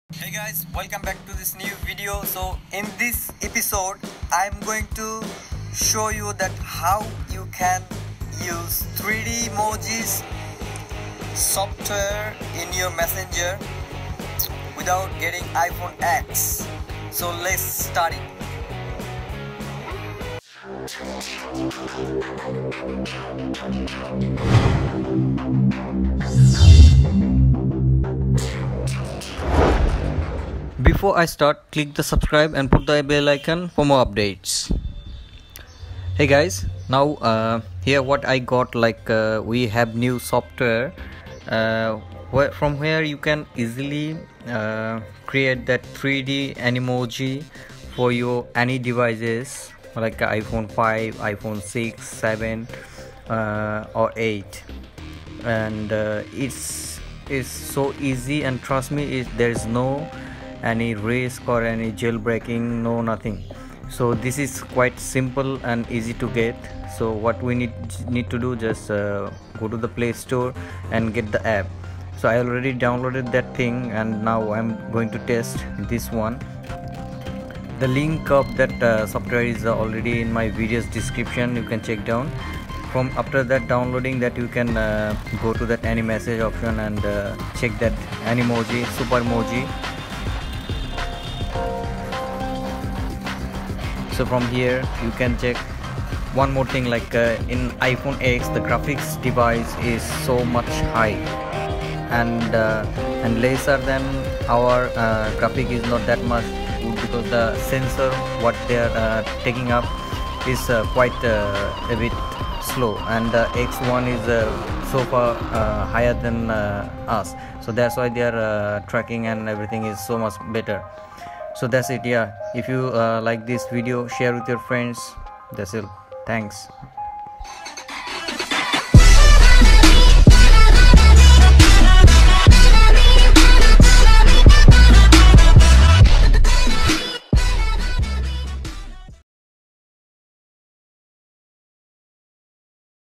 3D animoji Emoji for iPhone 5, 6, 7, 8 devices | SUPERMOJI & NO JAILBREAK | The sobuZ
Today I'm going to demonstrate to you how you can get Animoji for your iPhone 5s, 6 , 6s, 7, 8 and Plus/iPad and iPod on iOS 11 without the PC or escape or jailbreak.

Now here we go with new animoji app for any iPhone devices. It can call as SUPERMOJI on iTune store.

✱How to remove dark circle from iPhone back? side effect of open covers Tips & Tricks IOS 11.2.5